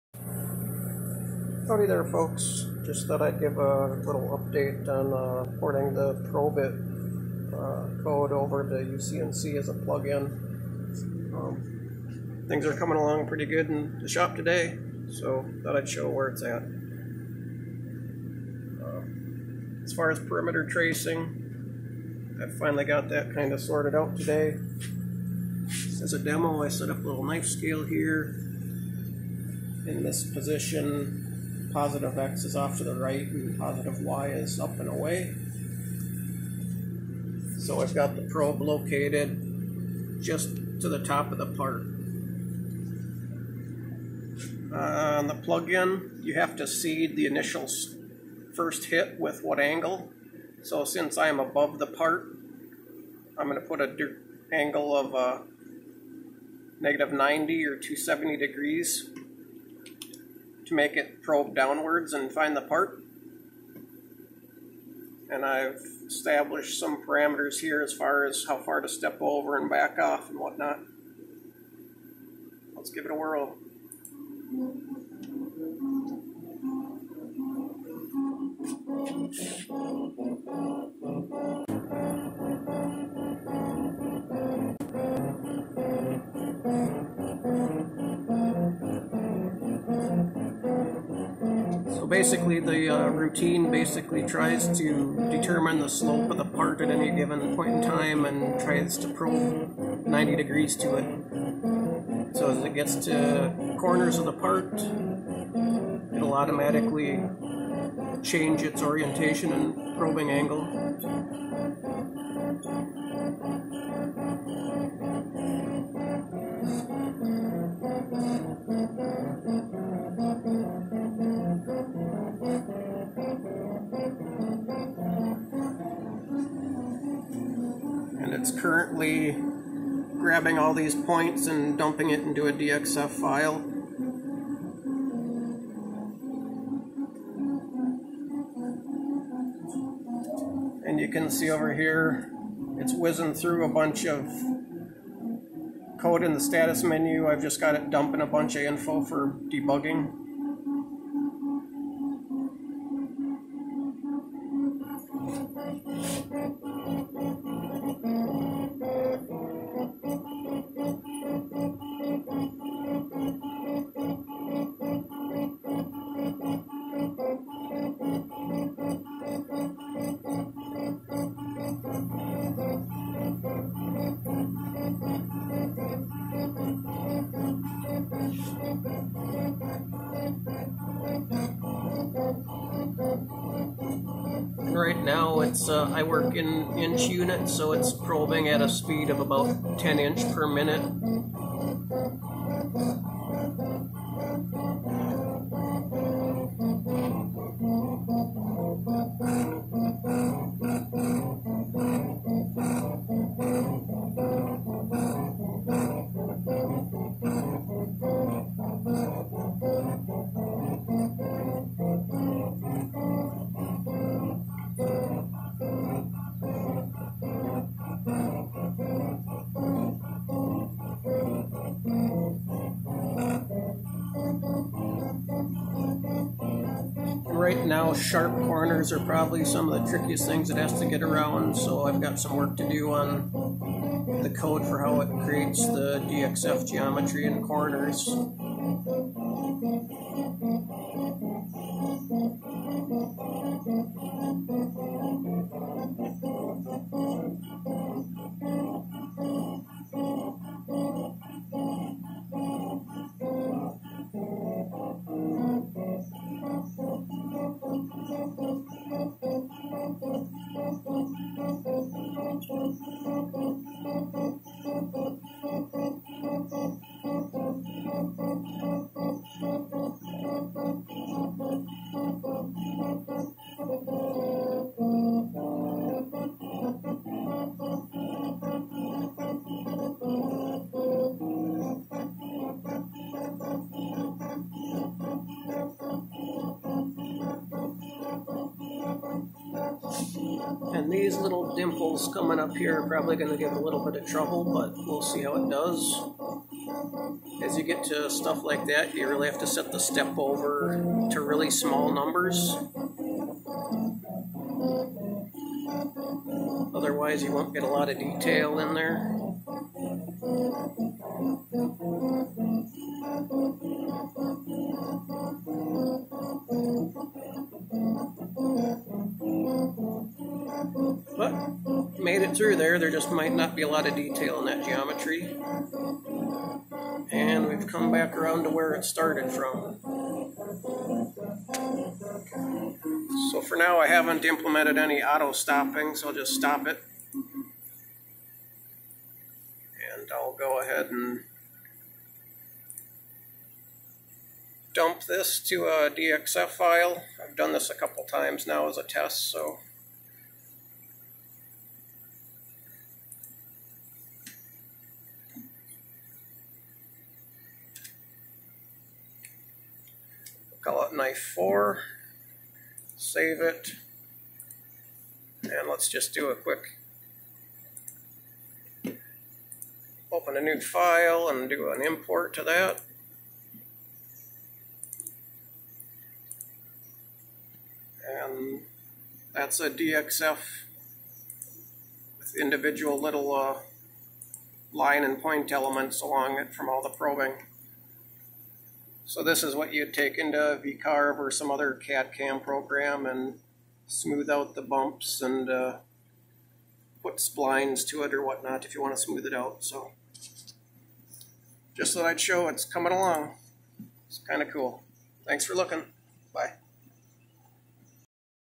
Uccnc version of ProbeIt perimeter testing
Progressing on the plugin to bring the 'ProbeIt' 2d tracing code to UCCNC.